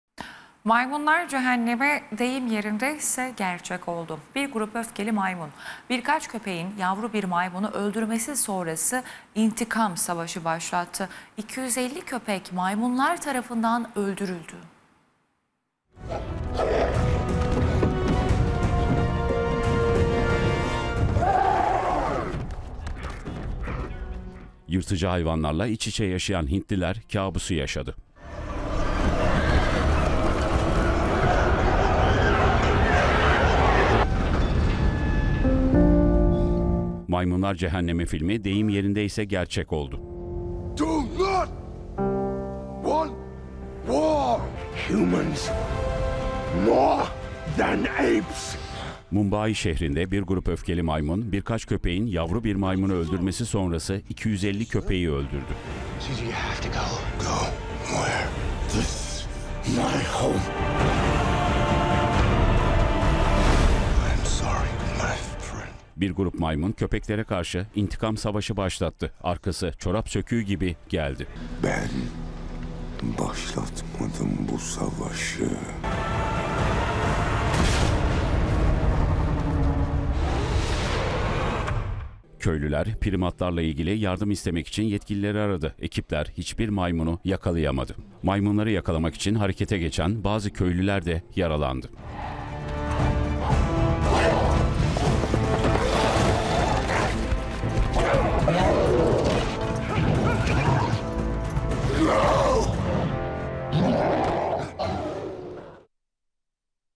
Hindistan'da İntikamcı Maymunlar 250 Köpek Öldürdü
Hindistan'ın ufak bir kasabasında maymunların, "intikam" için köpekleri öldürmeye başladığı iddia edildi.

TÜRKSAT 4A
Frekans: 12729
Sembol: 30000
Polarizasyon: V (Dikey)